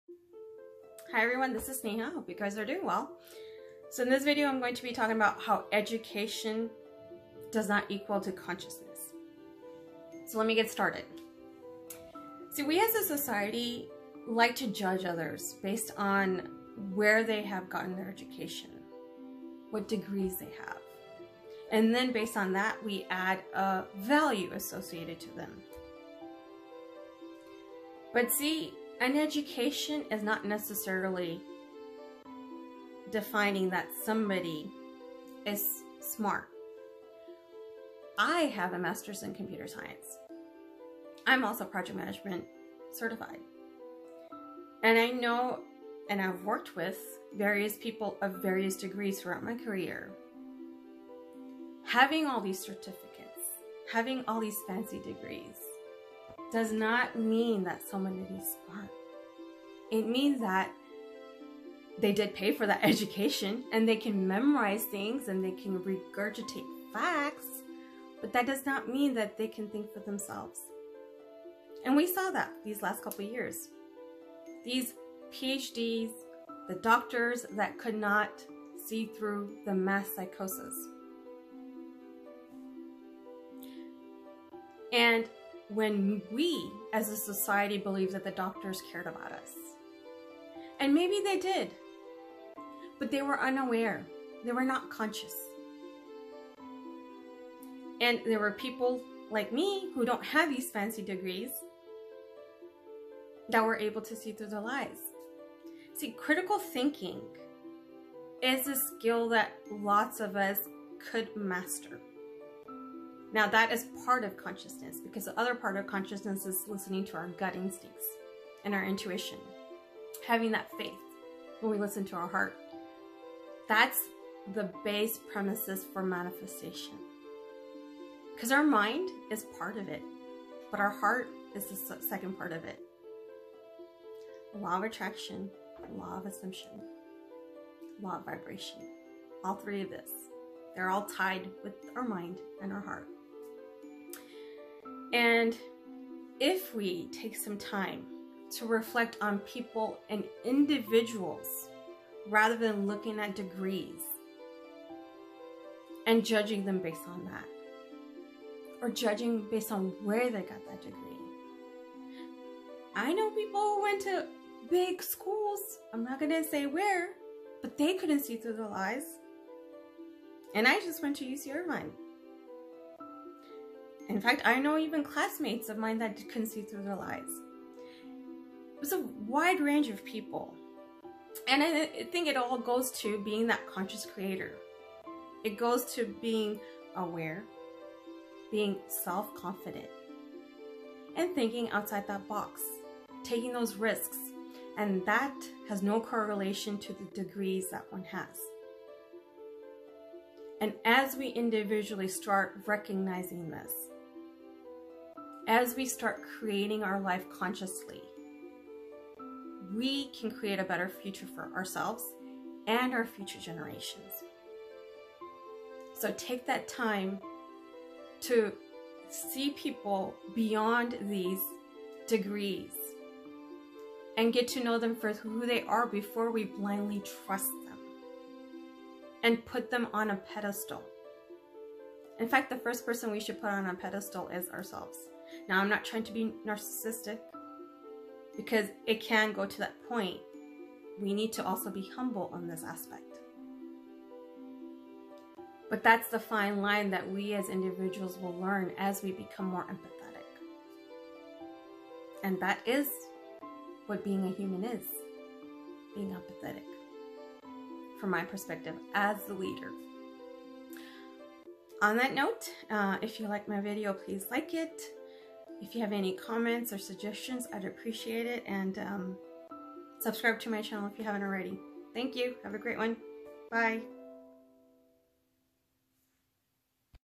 Education is not Wisdom
We as a society love to judge others.  Who doesn't? It helps us feel better temporarily.  Especially if we have more than them. Be it wealth, materials or education.  Just a few examples. This is a reminder that no matter how much education one gets that doesn't mean they have common sense. As someone who goes through both my hermit stages and those being extroverted, I can tell you mostly I like being a hermit when I'm tired of dealing with people who lack common sense.  I said it.  They are everywhere.  When one practices the pause before responding they'll realize what they were going to react to,  isn't really worth it.  Becoming more conscious and aware.

Pay attention.

Observe.

Listen.

Be empathetic.